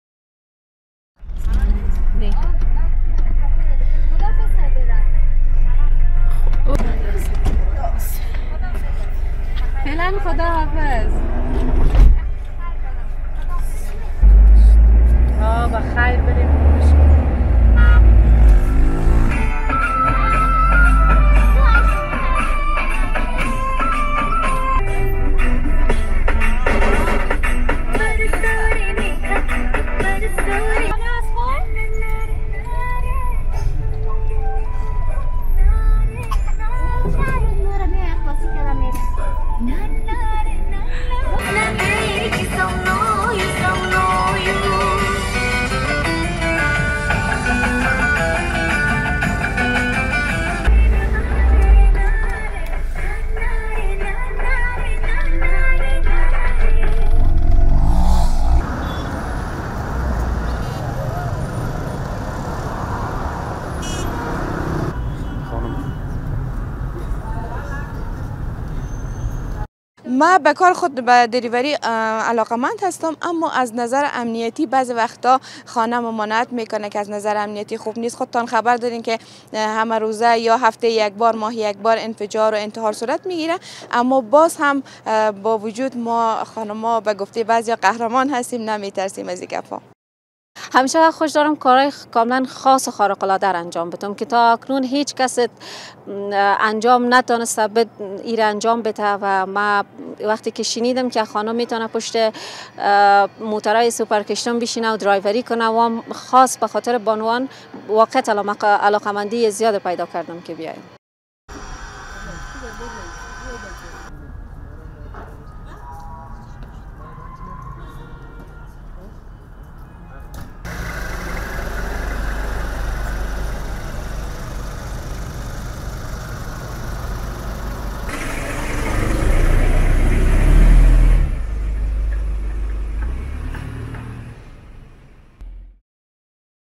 Afghan women turn to driving minivans in Kabul for public transportation
Razia Dalili, an Afghan female minivan driver, guides her minivan through Kabul's congested traffic, attracting astonished stares, smiles and expression of shock from other Afghans in an ultra-conservative society. [NAJIBA NOORI / AFPTV / AFP]